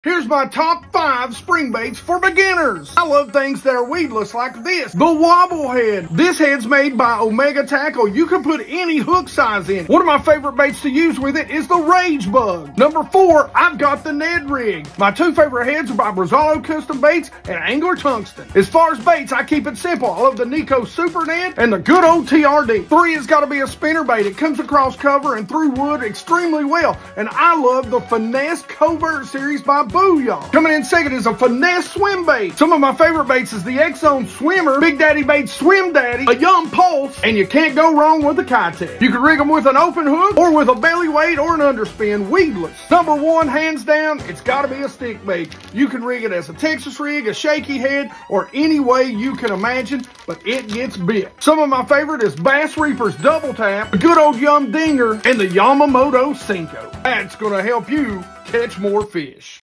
Top 5 Spring Baits For Beginners
These are some of the BEST baits for beginners. Especially in the spring. Here's my TOP 5 Baits for beginners fishing in the spring.